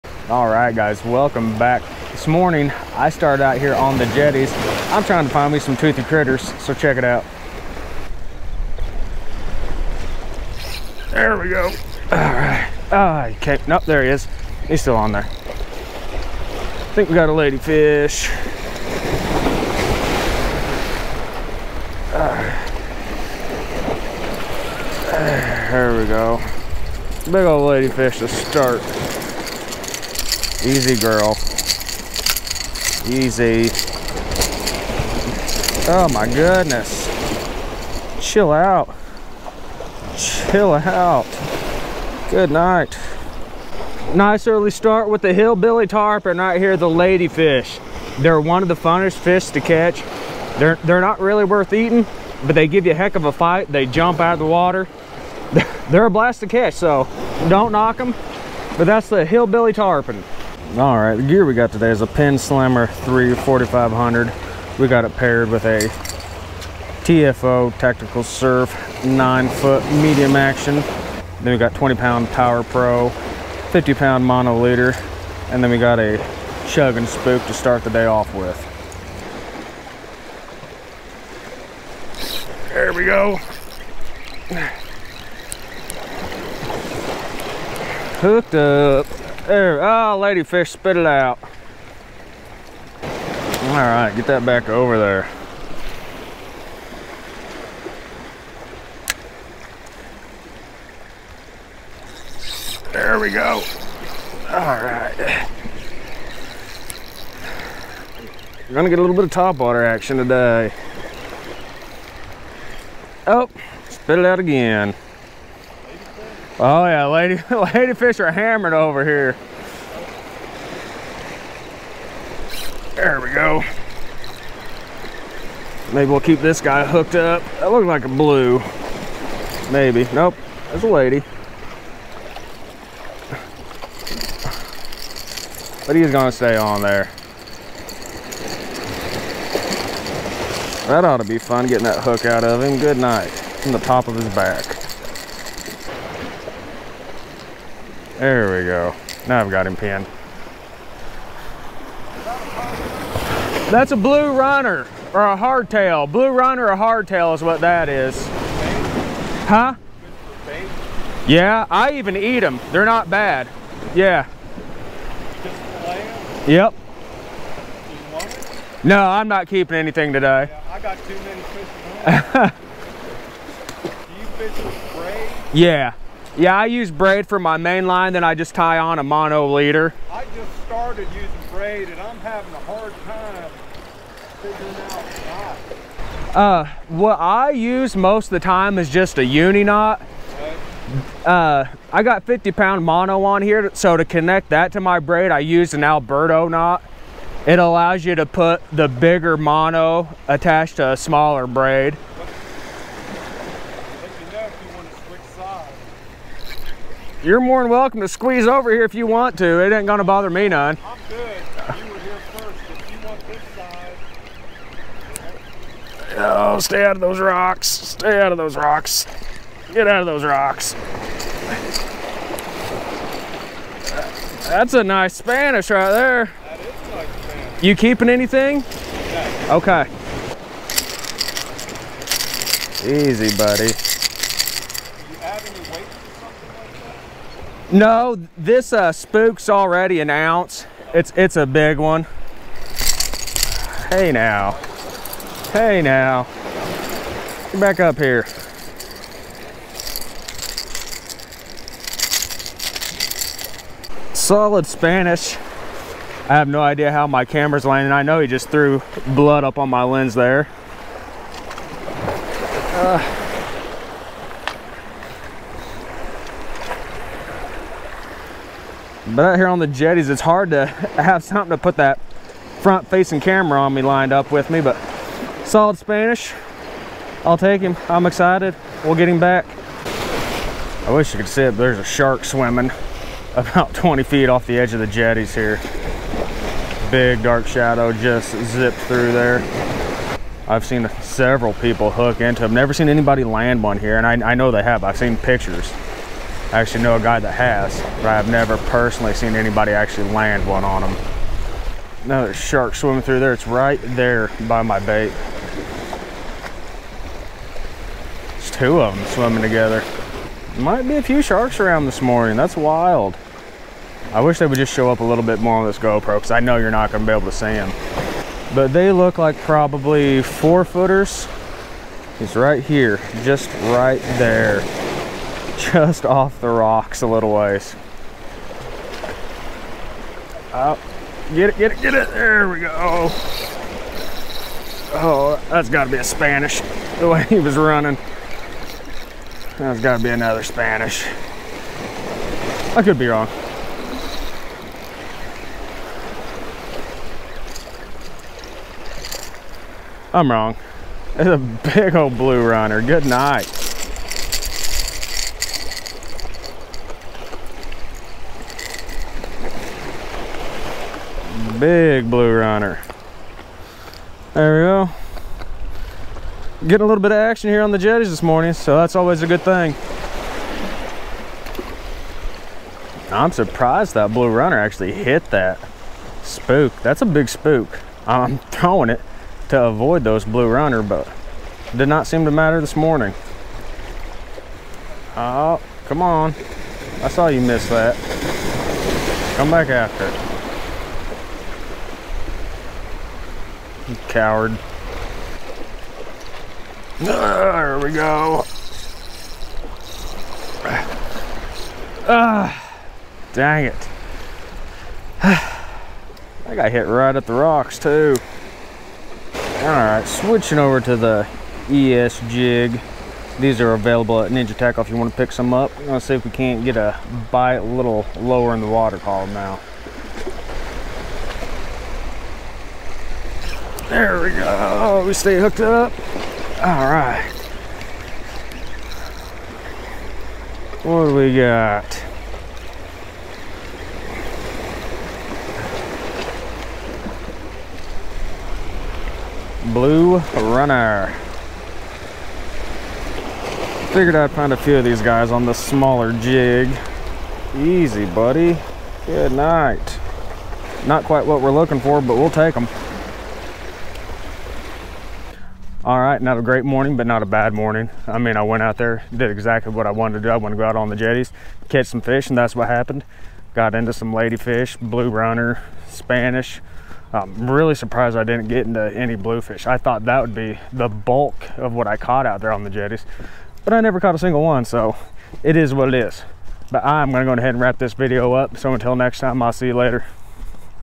Topwater Fishing Orange Beach Jetty
Topwater jetty fishing may be one of my new favorite things. The action wasn't crazy this day but I still had a blast on the jetty. Now I just gotta figure out how to get around the jetty a little more easily before I get hurt on them.